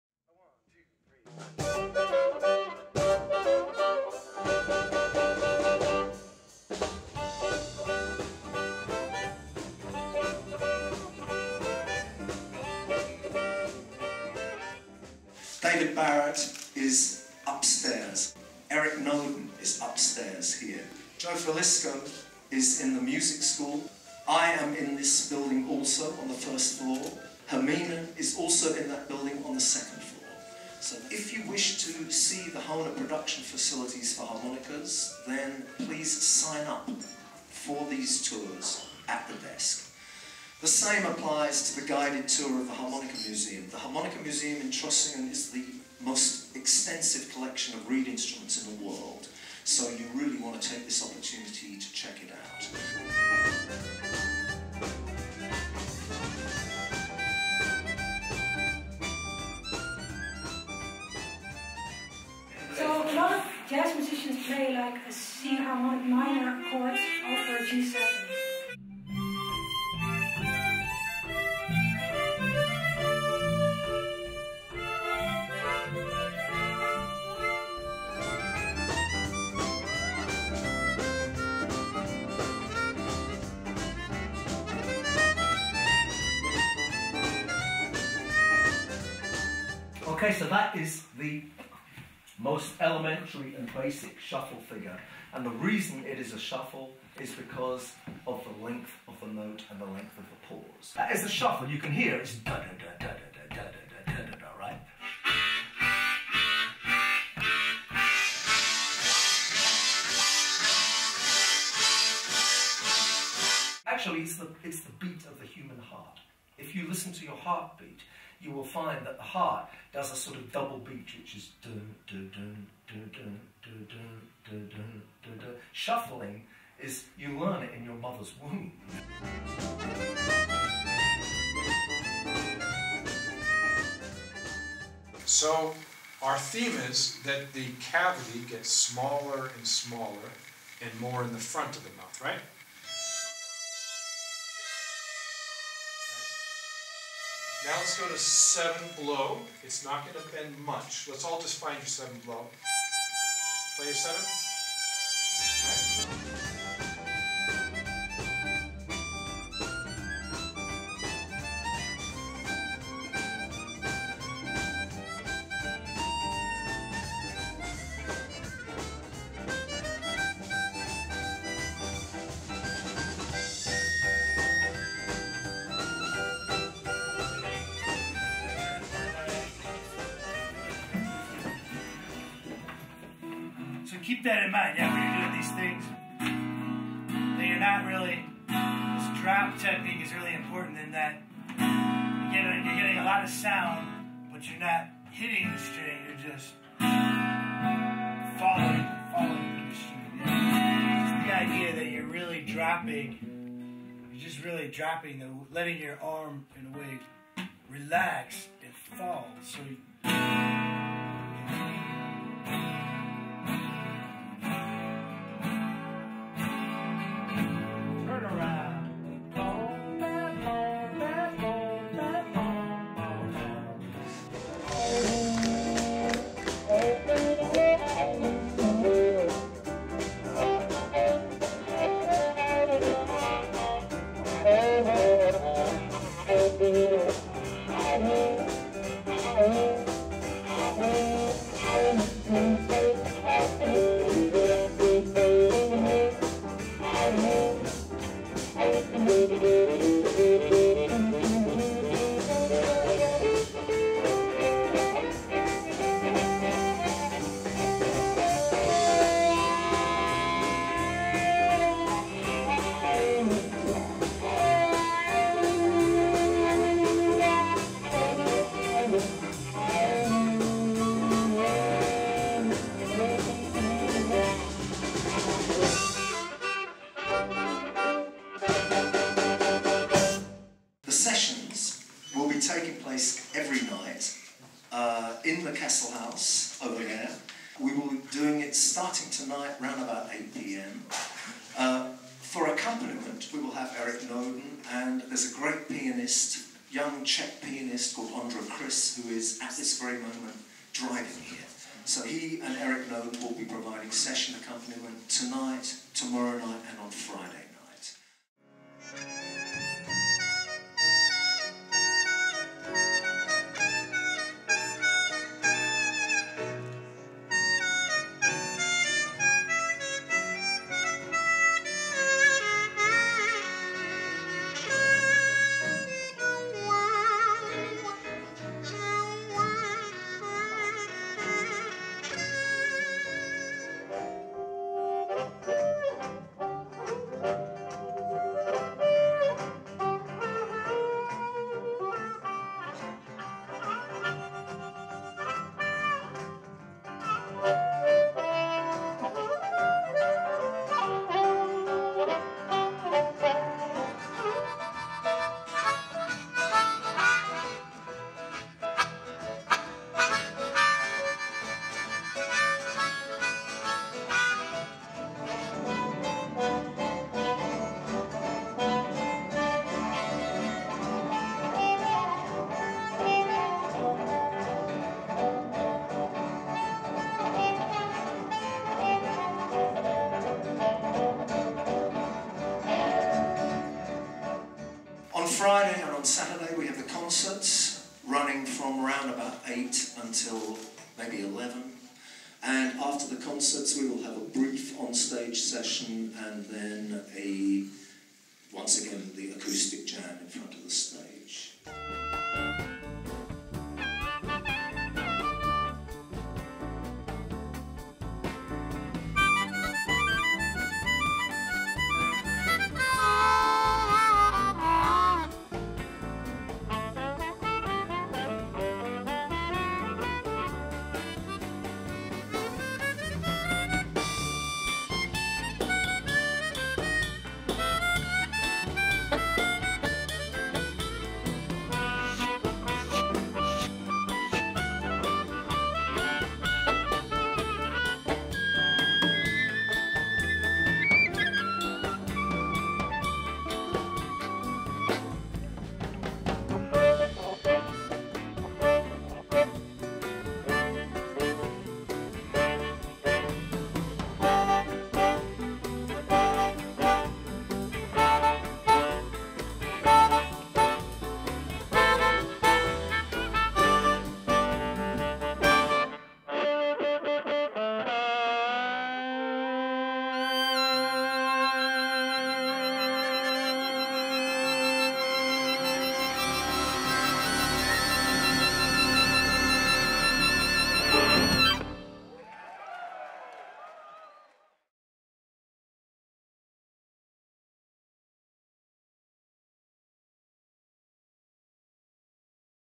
Harmonica Masters Workshops
The Harmonica Masters Workshops have been taking place in Trossingen since 2003. Initiated by musical director Steve Baker, with world-class instructors and a full supporting program of sessions and concerts, they are now regarded as the leading educational event for blues harmonica in  Europe.
Students at this truly international gathering make up a remarkable cross-section of European harmonica aficionados, plus visitors from both North and South America, the Far East and even Australia. As well as offering a great learning experience, the Harmonica Masters Workshops also provide the opportunity to get to know fellow harmonica enthusiast from many countries, to meet old friends and make new ones.
Don't miss this extraordinary event!
Trossingen, October 29th - November 2nd 2014